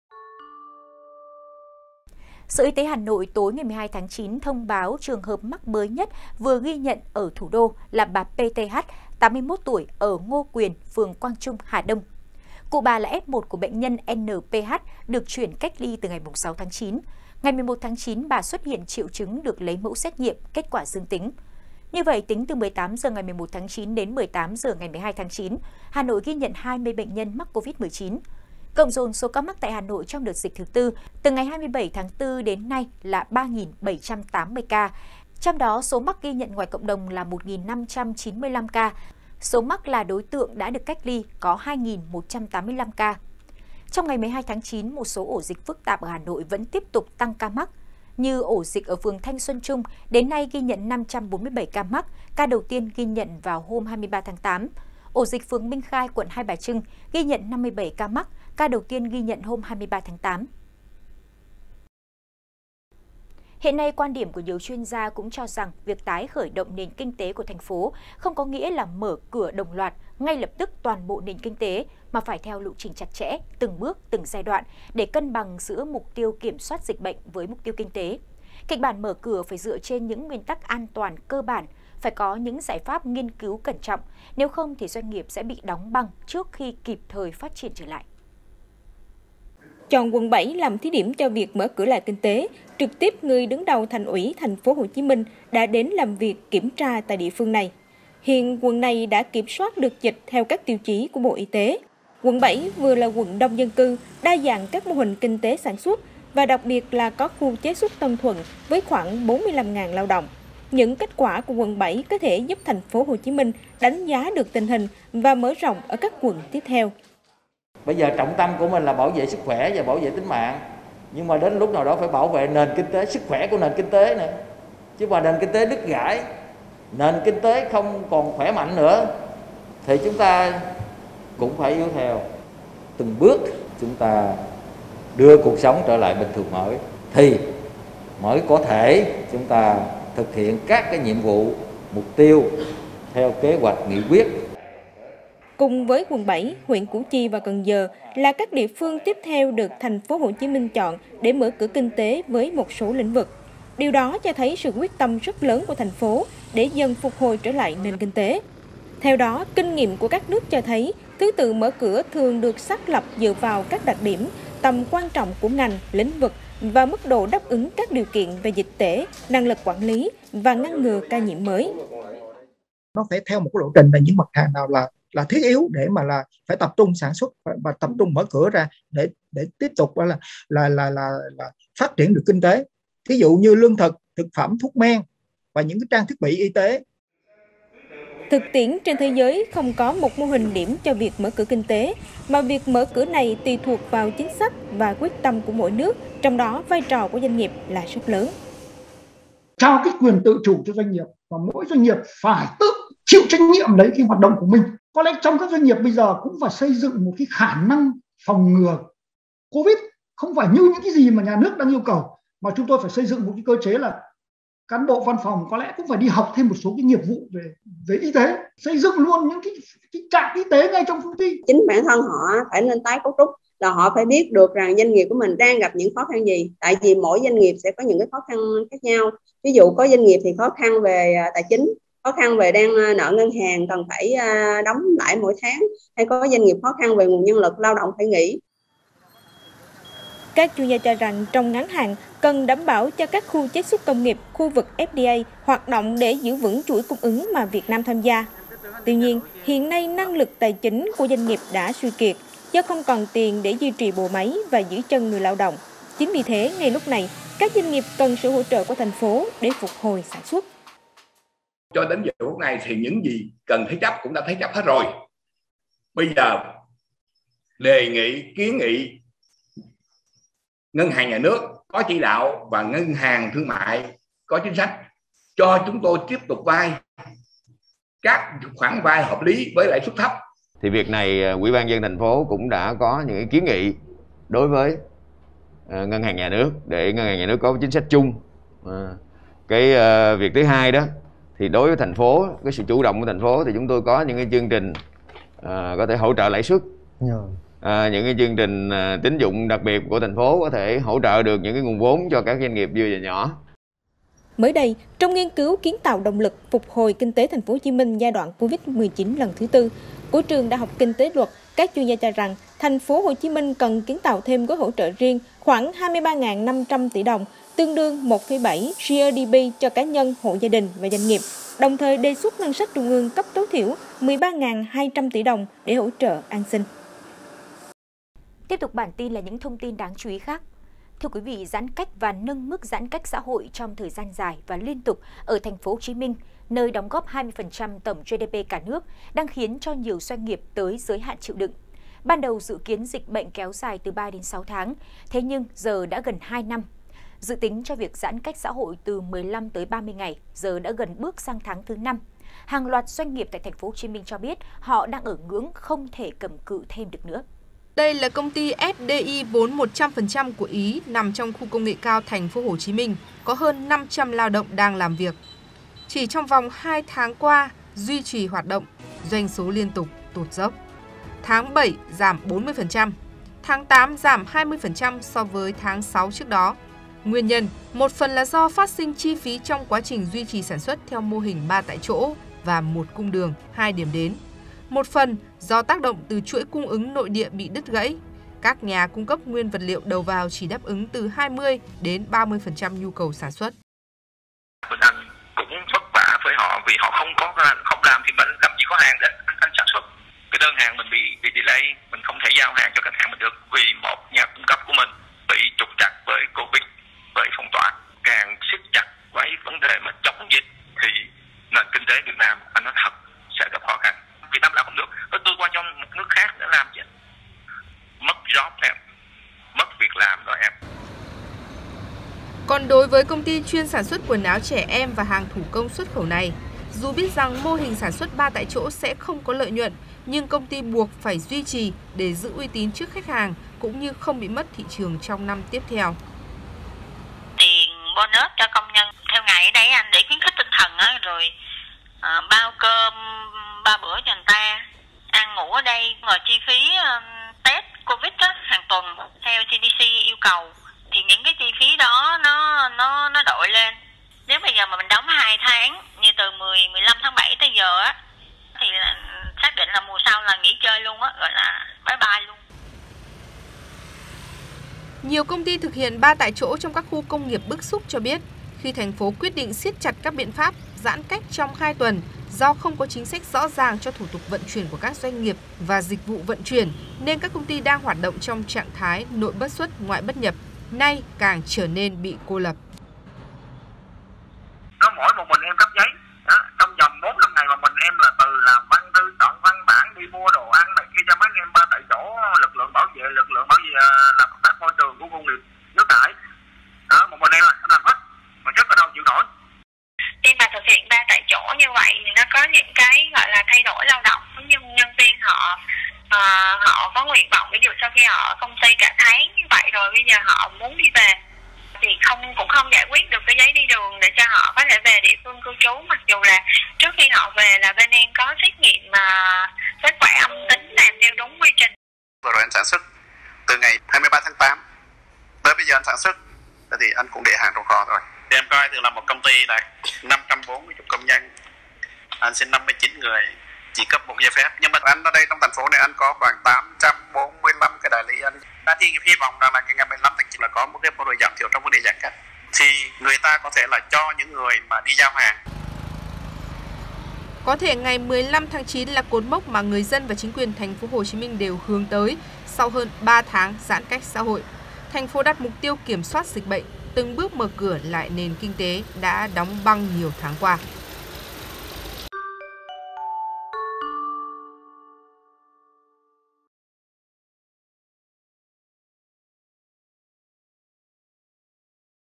VTC Tin mới
Tính từ 18h ngày 11/9 đến 18h ngày 12/9, Hà Nội ghi nhận 20 ca dương tính SARS-CoV-2, trong đó có 4 ca cộng đồng.